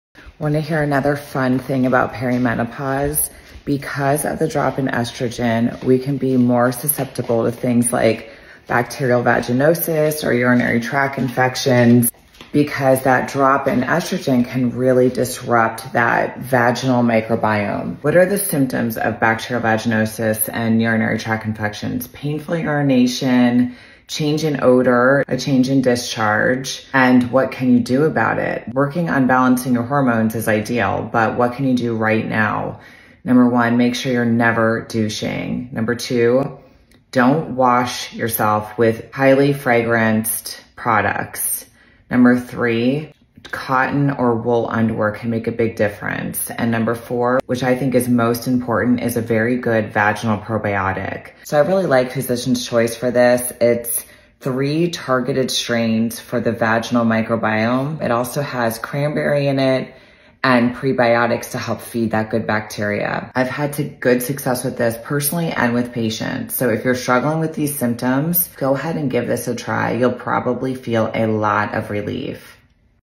Bacterial vaginosis and UTIs
Perimenopause comes with some challenges, that’s for sure. One that you may not realize can be related is an imbalance in the good to bad bacteria in the vaginal microbiome.

Things like bacterial vaginosis and urinary tract infections can start to become more common.

This probiotic is specifically targeted for the vaginal microbiome.

You do not have to be in perimenopause to have a UTI or BV, it can just become more common in the perimenopausal years.

code BECKY35